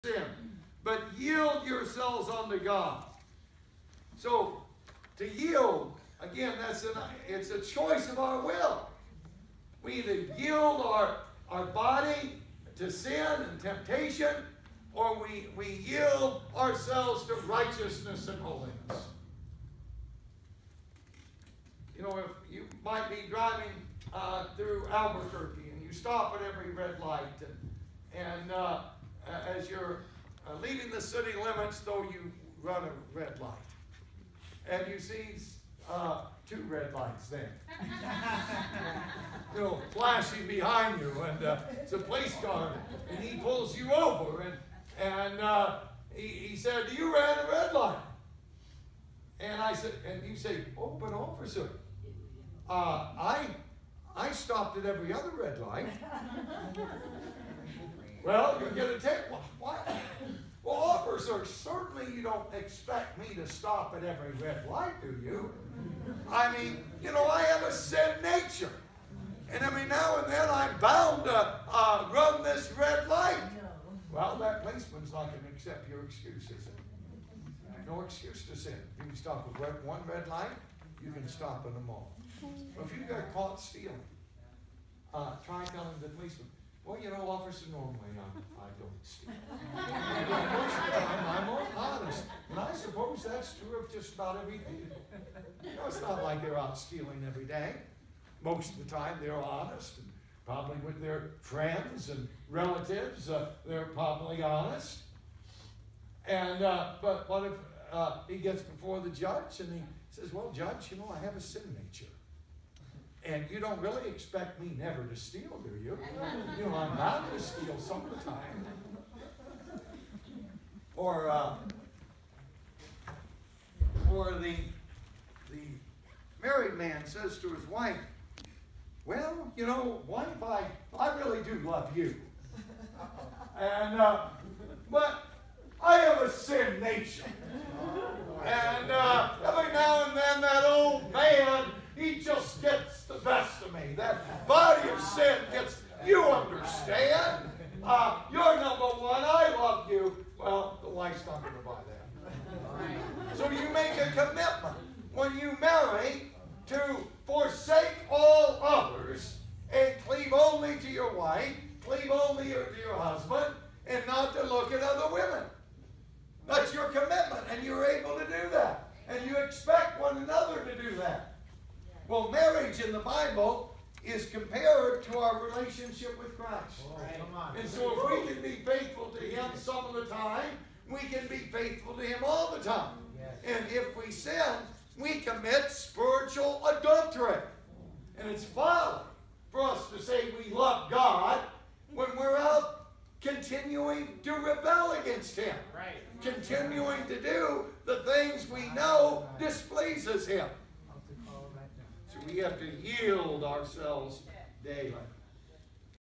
Just One Sin! Bro. Jed on Romans 6
Short excerpt from Bro. Jed preaching on Romans 6 at Praise Chapel Albuquerque